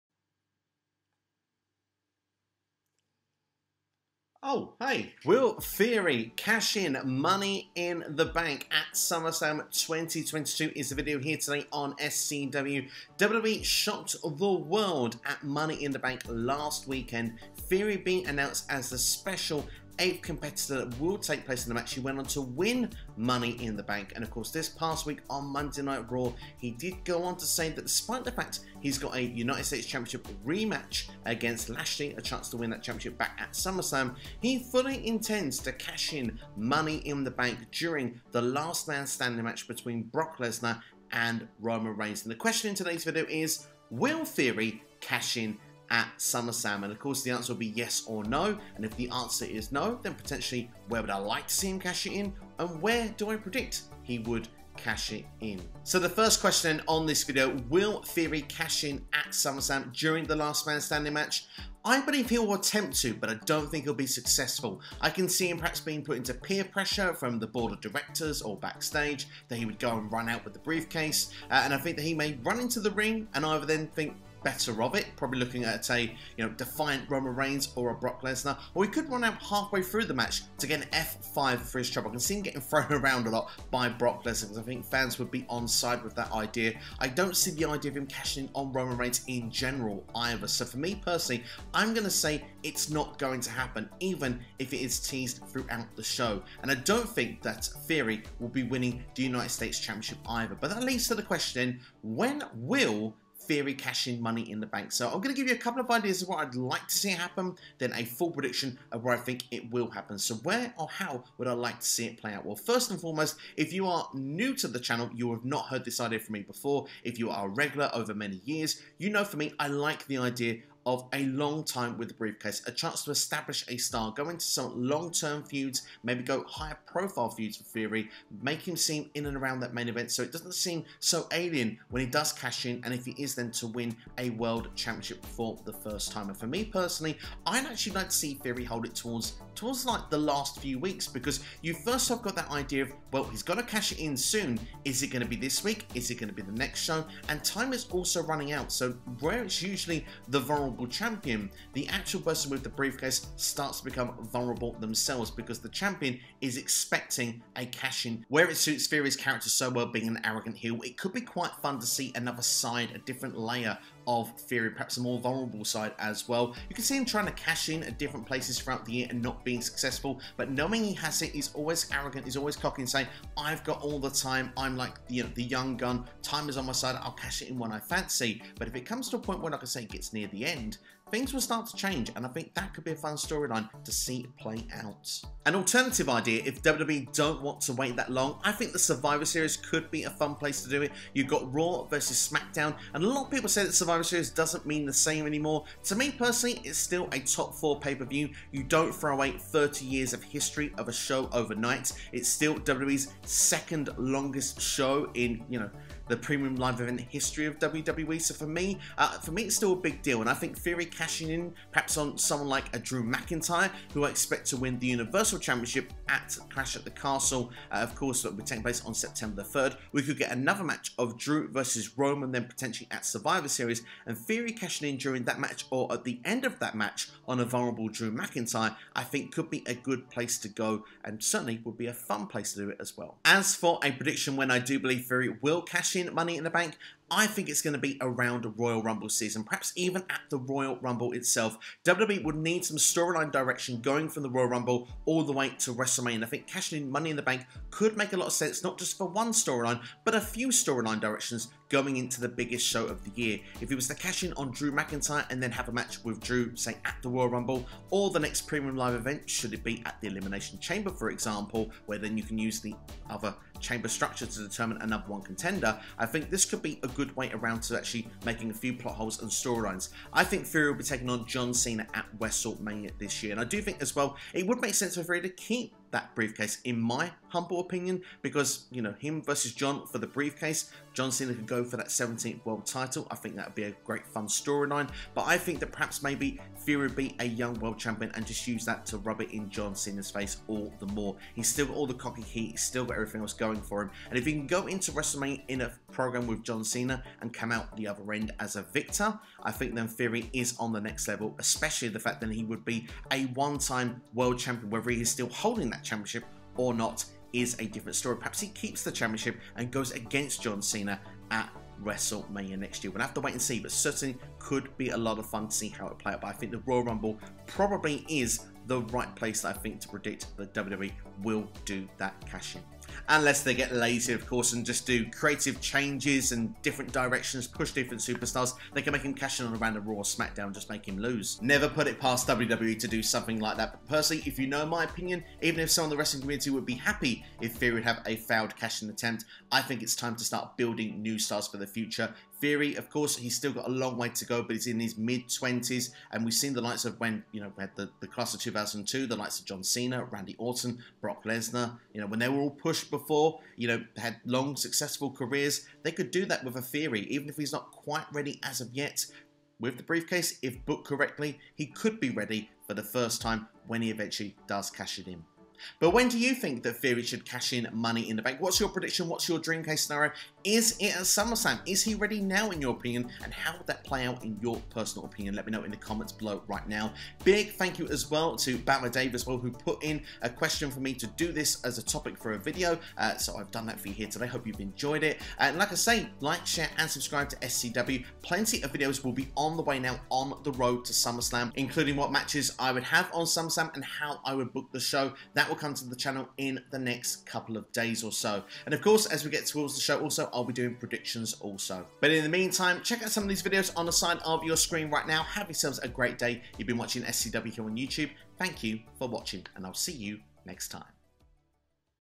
Will Theory Cash In Money In The Bank At WWE SummerSlam 2022?
Will Theory Cash In Money In The Bank At SummerSlam 2022?

If not where and when would you like to see him cash in MITB?

Chapters:
0:00 Intro
0:55 Will Theory Cash In Money In The Bank At SummerSlam 2022?
1:41 When I Would Like To See Theory Cash In His WWE MITB Briefcase
3:53 PREDICTION - When Will Theory Cash In Money In The Bank Briefcase?